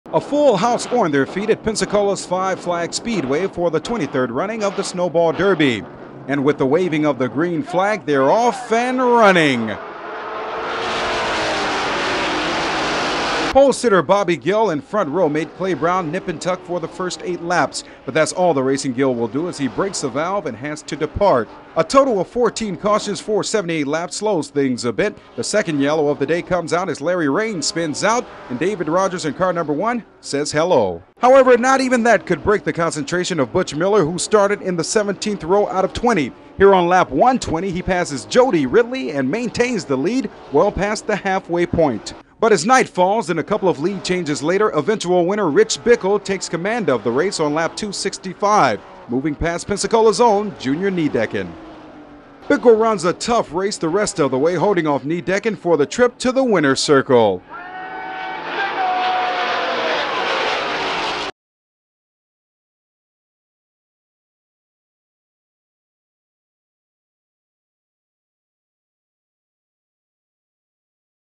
The 1990 Snowball Derby Highlights
Strategy played into the hands of Junior Niedecken who outlasted everyone except Rich Bickle, who powered by Niedecken for the win on lap 265 of the 332 laps completed that year.